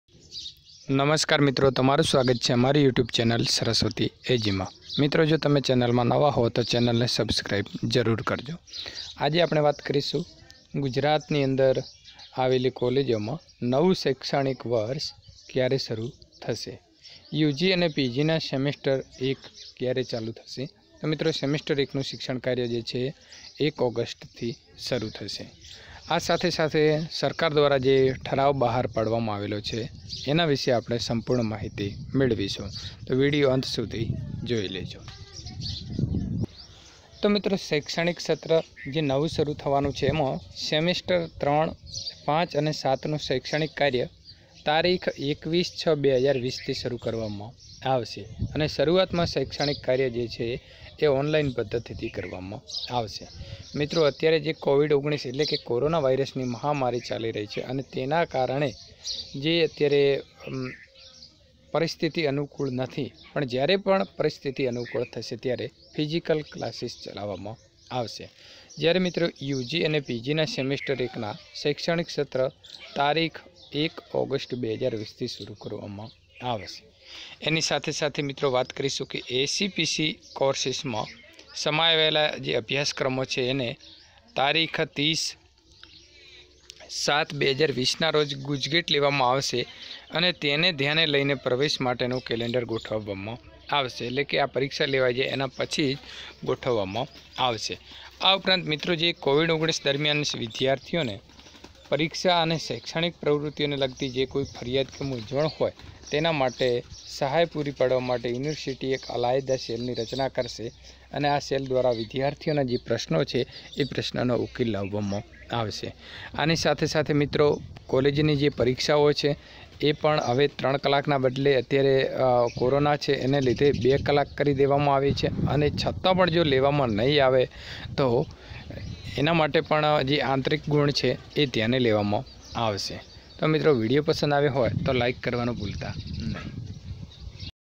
Collage exam 2020 gujarat , ગુજરાત સરકાર દ્વારા કોલેજ મા ક્યારે પરિક્ષા લેવામાં આવશે જાણો
h